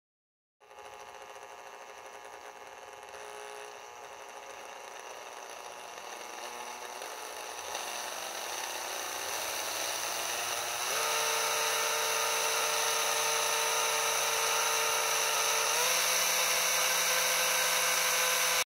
Bird Wing movement test for Grogamins workshop #1
chopstick prototype and handmade crankshaft to imitate wing movement
Grogamins 3d printed film props and animatronics